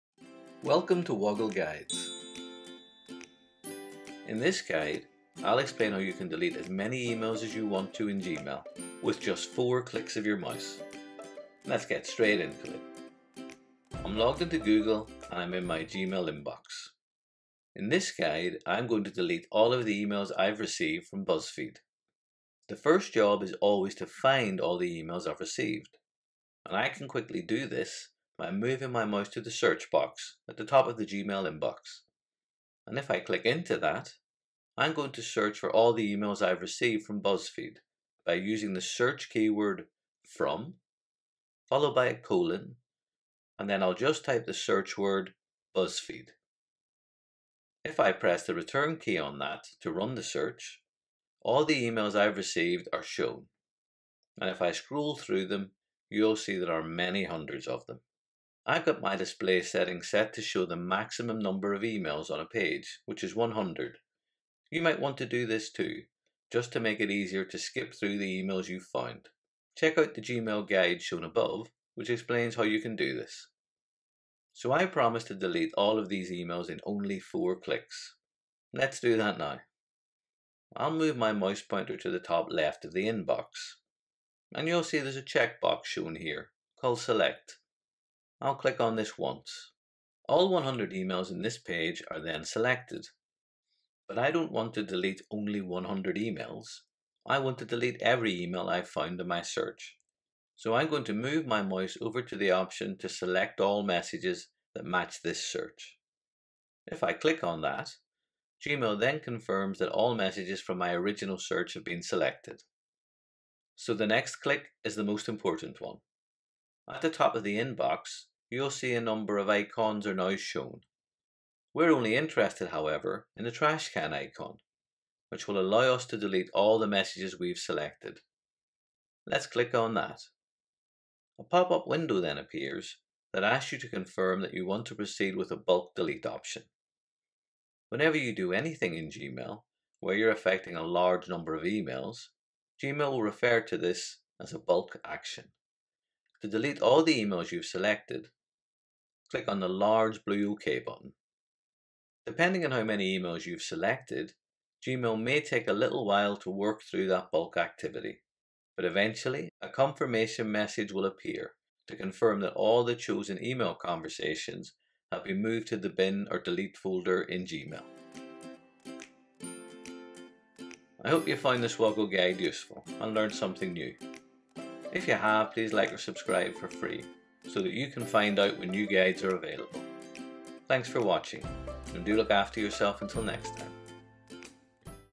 How to delete Gmail emails in bulk in only 4 clicks of your mouse
In this guide, I’ll explain how you can quickly delete thousands of emails in Gmail

🕔  Key Moments
00:00 | Introduction
00:15 | How to delete emails in bulk in Gmail
02:50 | Wrap up